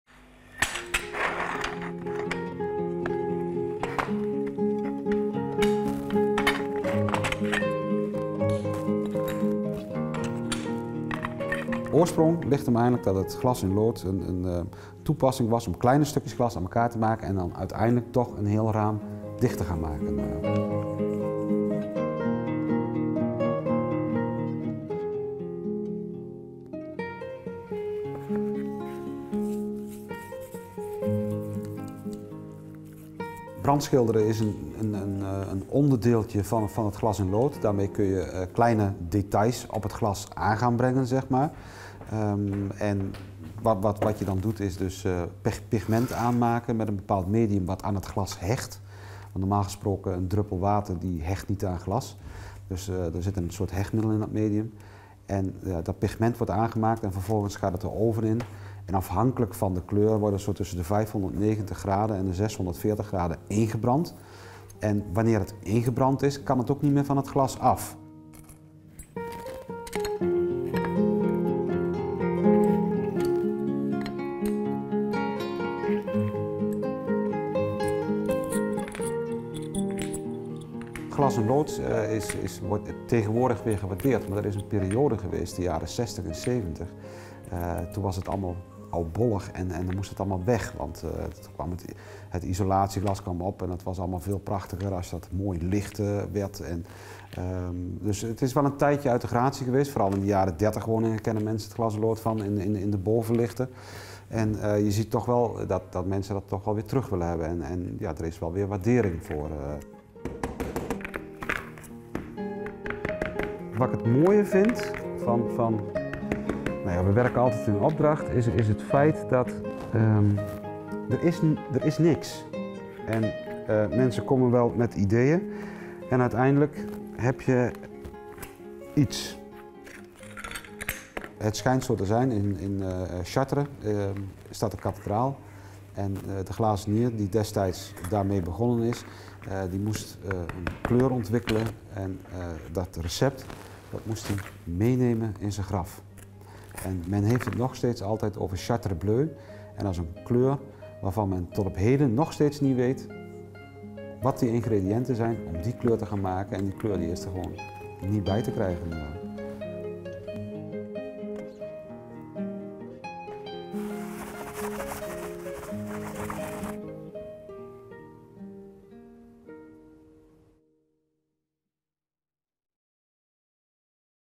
EWVV S3 Afl11 Glas in Lood 20160321140423
In onze serie ambachten hebben we vandaag aandacht voor het kleurrijke vak van glazenier Gerco Honders uit Bredevoort. Hij maakt in opdracht glas-in-lood-producten